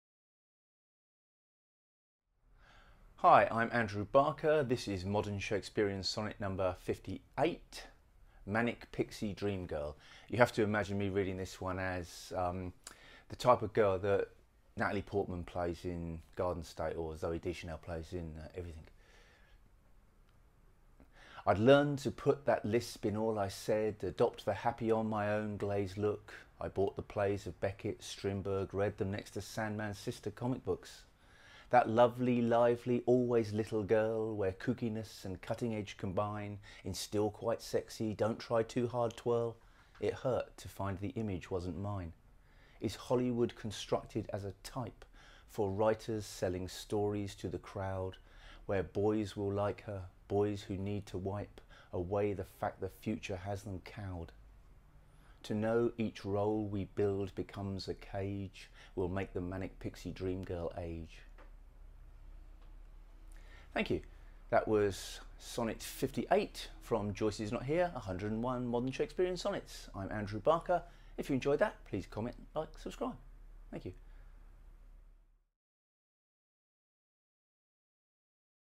Modern Shakespearean Sonnet 58. Manic Pixie Dream Girl, by Andrew Barker.
Andrew Barker's collection of original sonnets, "Joyce is Not Here: 101 Modern Shakespearean Sonnets,"  can be purchased, with a new cover, through Amazon at

Modern Shakespearean Sonnet 58. Manic Pixie Dream Girl.

I’d learnt to put that lisp in all I said,
Adopt the happy-on-my-own, glazed look.
I bought the plays of Beckett, Strindberg, read
Them next to Sandman’s sister comic books;
That lovely, lively, always little, girl
Where kookiness and cutting-edge combine
In still-quite-sexy, don’t-try-too-hard twirl:
It hurt to find the image wasn’t mine;
Is Hollywood constructed as a Type
For writers selling stories to the crowd,
Where boys will like her, boys who need to wipe
Away the fact the future has them cowed.
           To know each role we build becomes a cage
           Will make the Manic Pixie Dream Girl age.

                                                                                                Andrew Barker

Analysis.
On screen or in life I've never really trusted the affectation behind this type of self-presentation: It's the necessarily infantilizing aspect of the way the character behaves, I suppose.
On a side note, I don't like Beckett much either, but Strindberg's "Miss Julie" is wonderful, and Neil Gaiman's two Death comic books are great fun.
But let's face it, cheerful people are usually more fun to be around than their opposite, so the MPDG persona can't be all bad.
                                                                                                 Andrew Barker.